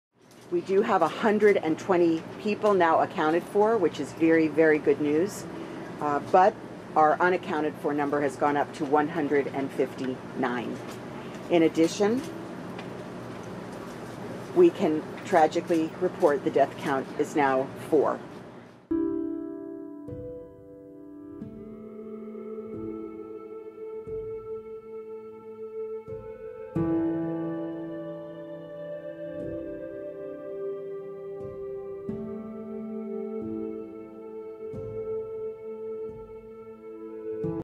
Four dead, 159 unaccounted for in Miami building collapse
The number of people unaccounted for following the collapse of a Florida apartment block has risen to 159 and the confirmed death toll to four, says Miami-Dade County mayor Daniella Levine Cava.